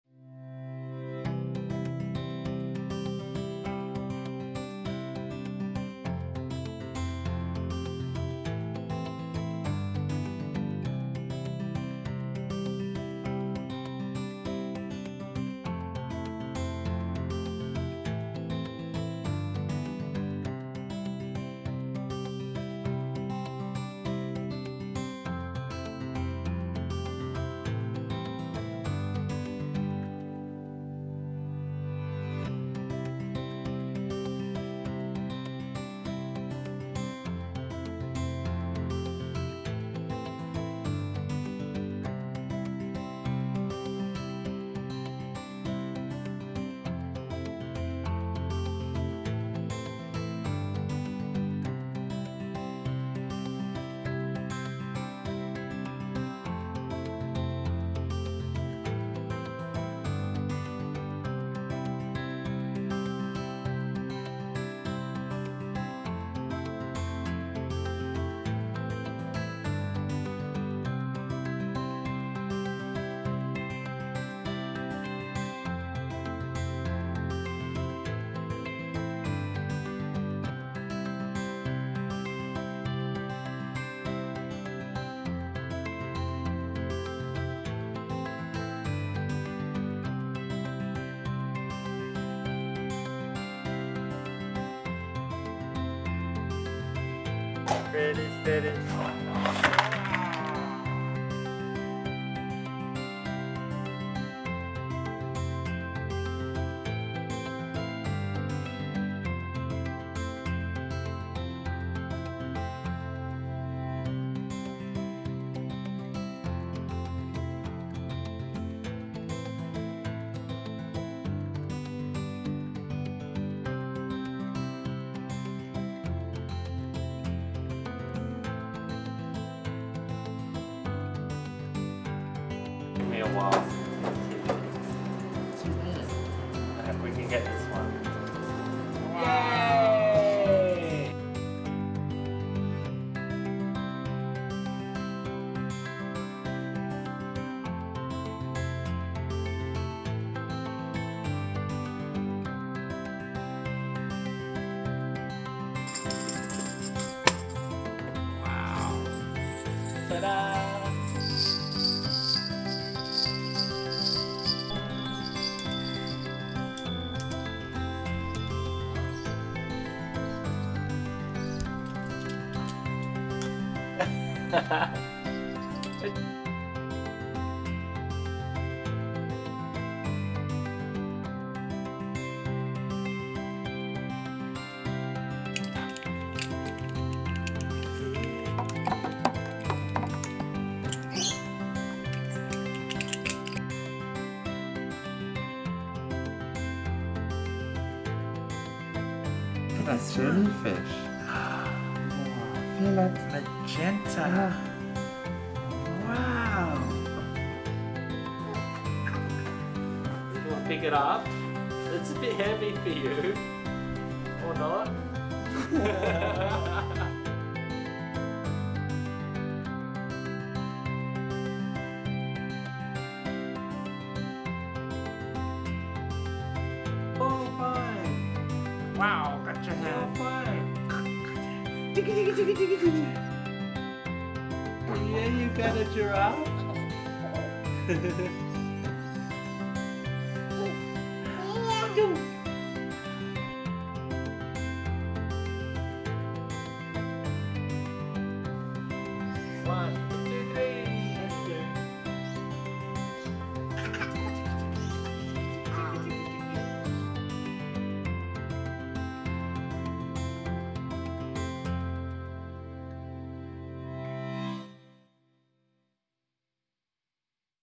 Documentary and Story Telling Photography and Film - Brisbane Photographer - Pedro Palma
Another beautiful family that I had the pleasure to work with. They kindly let me share their film with you to demonstrate how important it is to create memories that we can cherish for life. My love and appreciation goes out to them.